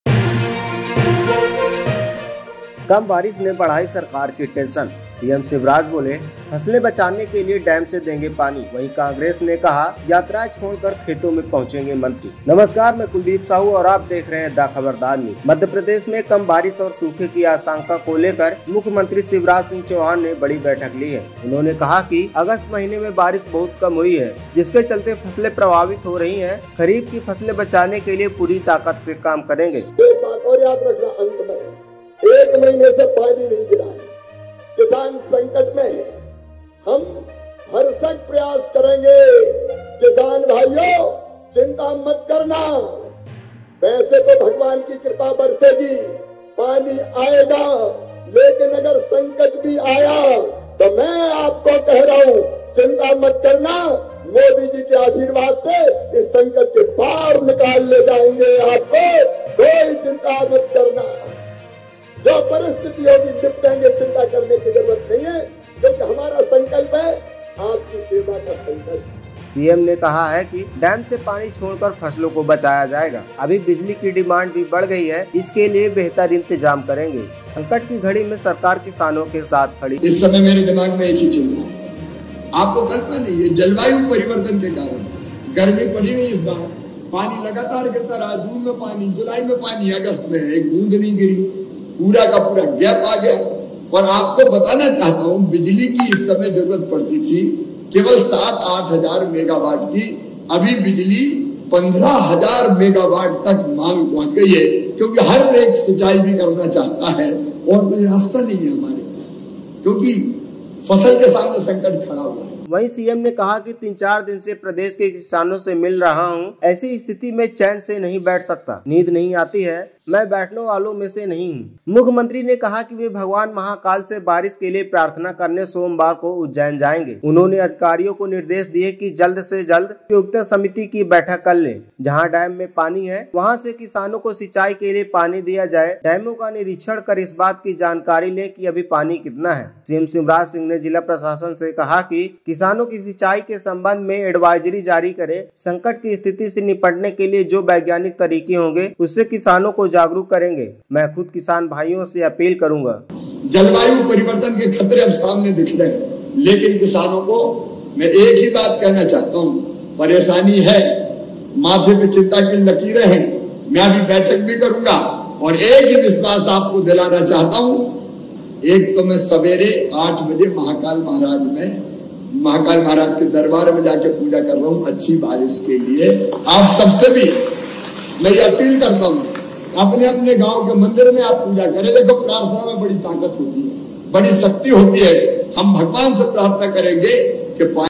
CM शिवराज बोले-फसलें बचाने डैम से देंगे पानी; कांग्रेस ने कहा यात्राएं छोड़ खेतों में पहुंचें मंत्री
मध्यप्रदेश में कम बारिश और सूखे की आशंका को लेकर मुख्यमंत्री शिवराज सिंह चौहान ने बड़ी बैठक ली। उन्होंने कहा कि अगस्त महीने में बारिश बहुत कम हुई है। जिसके चलते फसलें प्रभावित हो रही हैं। खरीफ की फसलें बचाने के लिए पूरी ताकत से काम करेंगे। सीएम ने कहा कि डैम से पानी छोड़कर फसलों को बचाया जाएगा। अभी बिजली की डिमांड बढ़ गई है। इसके लिए बेहतर इंतजाम करेंगे। संकट की घड़ी में सरकार किसानों के साथ खड़ी है।
सीएम ने कहा कि तीन चार दिन से प्रदेश के किसानों से मिल रहा हूं। ऐसी स्थिति में चैन से नहीं बैठ सकता। नींद नहीं आती है। मैं बैठने वालों में से नहीं हूं। प्रार्थना में बड़ी ताकत होती है। मुख्यमंत्री ने कहा कि वे भगवान महाकाल से बारिश के लिए प्रार्थना करने सोमवार को उज्जैन जाएंगे।
सिंचाई के लिए डैम से दिया जाएगा पानी
उन्होंने अधिकारियों को निर्देश दिए कि जल्द जल उपयोगिता समिति की बैठक कर लें। जहां डैम में पानी है, वहां से किसानों को सिंचाई के लिए पानी दिया जाए। डैमों का निरीक्षण कर इस बात की जानकारी ले लें कि अभी कितना पानी है। सीएम शिवराज सिंह ने जिला प्रशासन को कहा कि किसानों को सिंचाई के संबंध में एडवाइजरी जारी करें। संकट की स्थिति से निपटने के लिए जो वैज्ञानिक तरीके होंगे उससे किसानों को जागरूक करेंगे। मैं खुद किसान भाइयों से अपील करूंगा।
किसानों ने कहा, पर्याप्त बिजली नहीं मिल रही है...
किसानों ने कांग्रेस नेताओं को बताया कि उन्हें पर्याप्त बिजली नहीं मिल रही है। मात्र 4 से 5 घंटे बिजली मिलती है। सूखे जैसे हालात बनने और पर्याप्त बिजली नहीं मिलने के चलते सोयाबीन की फसल खराब हो चुकी है। कम बिजली आने के चलते सिंचाई का भी कोई उपयुक्त साधन नहीं बन पा रहा है। जिससे सोयाबीन की फसल सूख गई है।
कांग्रेस के राज्यसभा सांसद विवेक तन्खा और विधायक कुणाल चौधरी ने शाजापुर के कालापीपल क्षेत्र में खेतों में जाकर फसलें देखी। कांग्रेस नेताओं ने खेतों में पहुंचकर सोयाबीन की खराब हुई फसलों को देखा।
कांग्रेस विधायक कुणाल चौधरी ने आरोप लगाया कि मुख्यमंत्री सिर्फ इवेंट की पॉलिटिक्स करते हैं। सूखे के कारण किसान की हालत खराब है। गेहूं में भी यही स्थिति की। जब भाव बढ़ रहे थे तो निर्यात बंद कर दिया। प्याज के भाव बढ़ रह थे तो निर्यात बंद कर दिया।